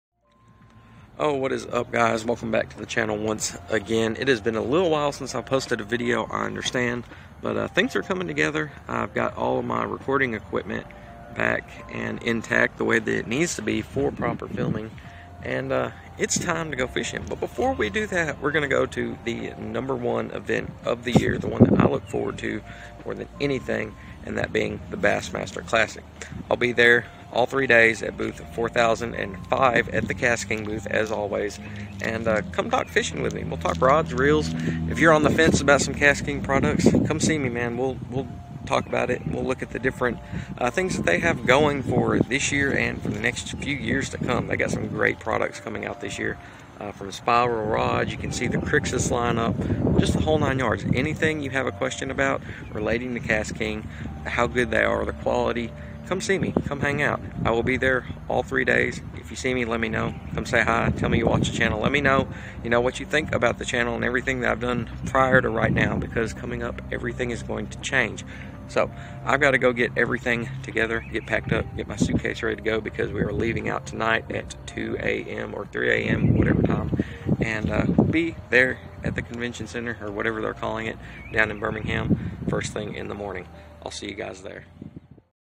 Come Hang Out with ME at the 2020 BASSMASTER CLASSIC!!!
Yo guys, if you're in the Birmingham Alabama Area March 6th-8th, come on out to the Bassmaster Classic Expo! I'll be at booth #4005 with KastKing all Weekend! i cant wait to see your faces 👊